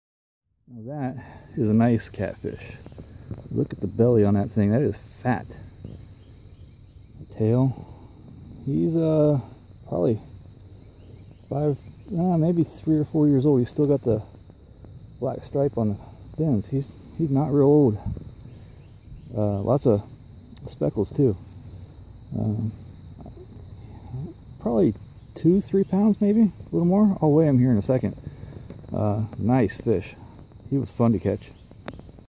Biggest Channel Catfish of the Year
Fishing at Lake Loramie State Park on the northwest creek channel. Caught about 15 catfish for the day, but this was by far the biggest prize